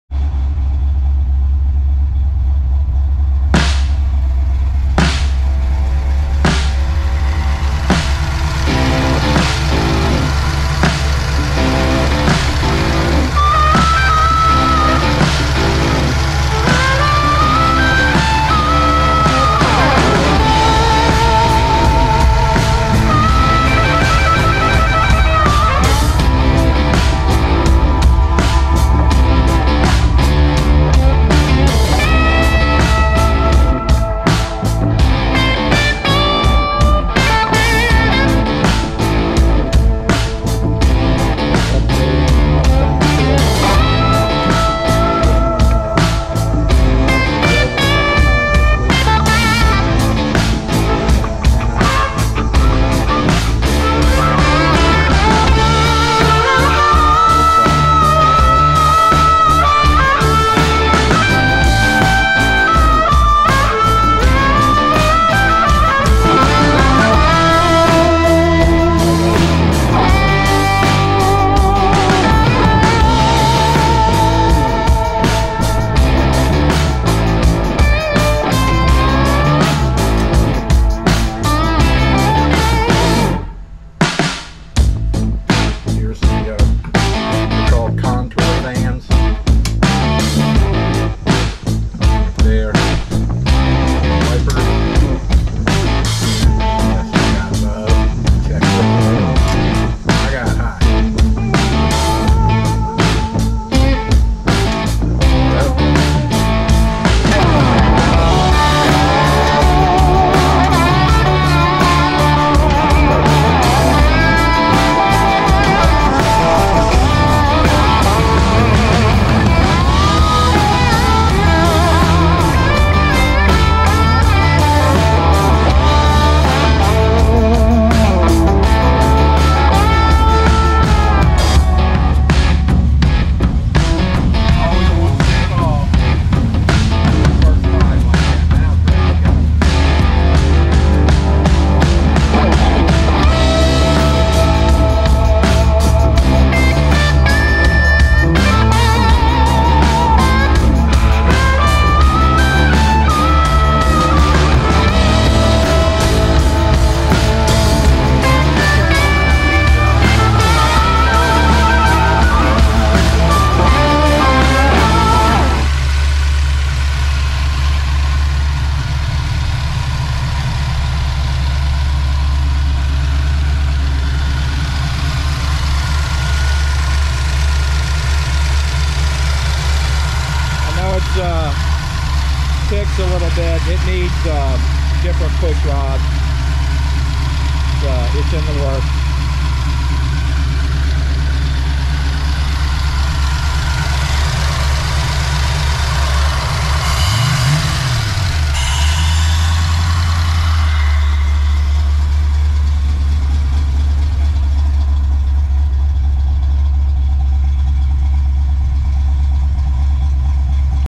89 LX Notchback AOD 5.0, 88 Mustang GT 5.0 T-5
Both cars have air conditioner deleted, one wire alternators (Summit Racing 140 amp), longtube headers, aluminum radiators with twin fans (Mishimoto 89, Contour 88) 89 has true dual exhaust with flowmasters, 88 has an h-pipe with Mac pro dumps & Holley Terminator X Kit installed with 6.86 Holley Digital Dash! Both cars have Trick Flow stage 2 cams.